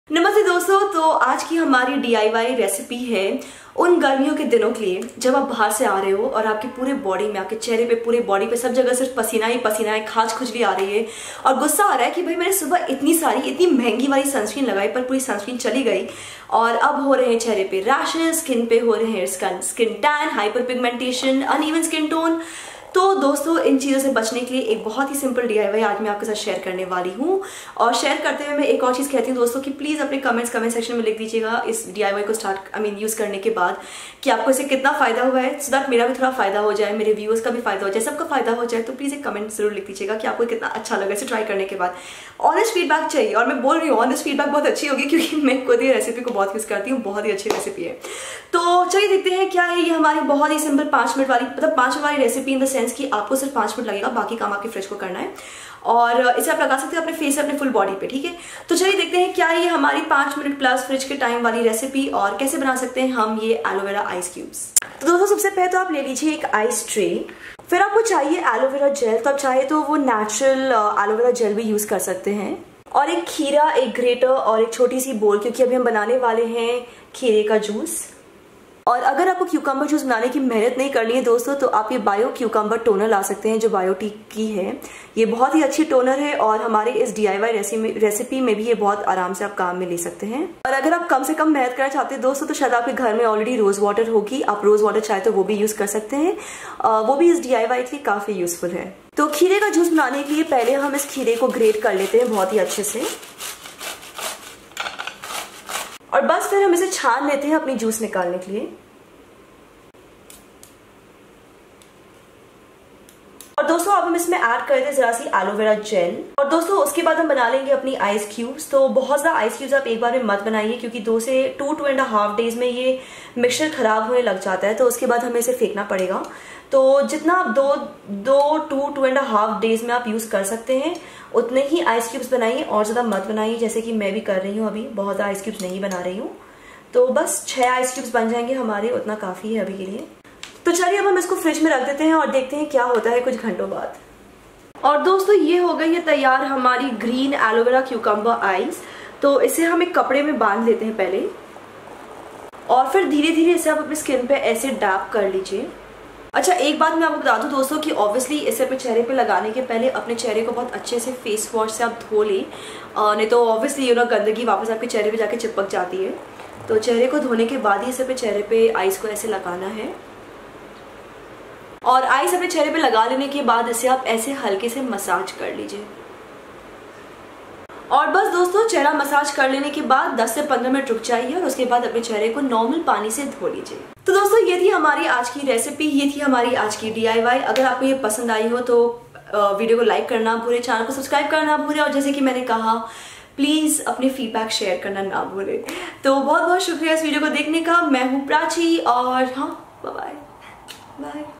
Hindi Video: DIY _ Aloe Vera Ice Cubes For Summer Skin Care
Aloe vera and cucumber ice cube - summer skin care DIY for instant skin repair.

Use these aloe vera ice cubes this summer to prevent sun tan, prevent skin rash and stop pimples in summer.